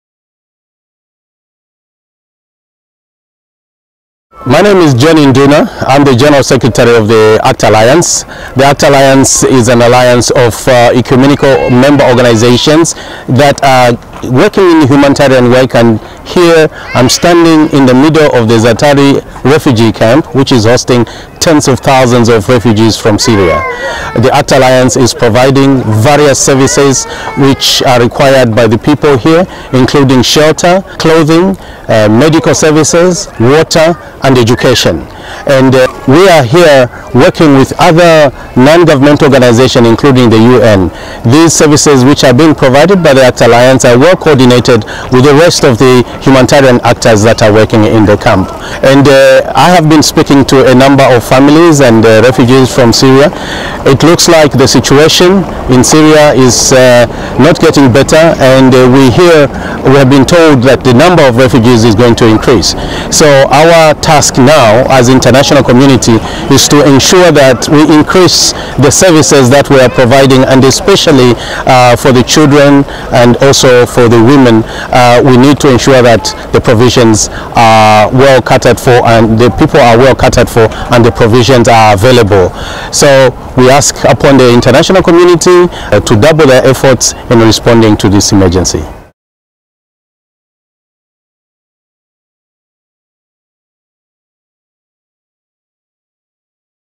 Opvang Syrische vluchtelingen in het Zaatari vluchtelingenkamp in Jordanië
Opvang Syrische vluchtelingen in het Zaatari vluchtelingenkamp in Jordanië waar Kerk in Actie samenwerkt met de ACT Alliance. In deze film vertelt de algemeen secretaris van ACT hierover. Deze film is vertoond tijdens de Landelijke Diaconale Dag van Kerk in Actie op 9 november 2013 in het Beatrixtheater in Utrecht.